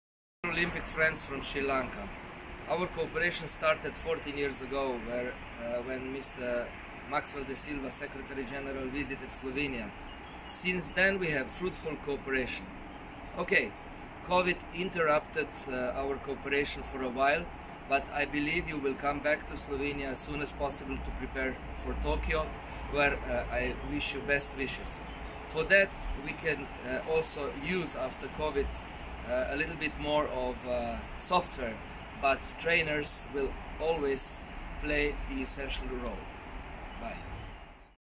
𝐎𝐥𝐲𝐦𝐩𝐢𝐜 𝐃𝐚𝐲 𝟐𝟎𝟐𝟎 𝐌𝐞𝐬𝐬𝐚𝐠𝐞 𝐟𝐫𝐨𝐦 𝐌𝐫. 𝐃𝐚𝐦𝐣𝐚𝐧 𝐏𝐢𝐧𝐭𝐚𝐫
A message from Mr. Damjan Pintar, Olympic Solidarity external expert for sport, for the Olympic Day Celebration 2020.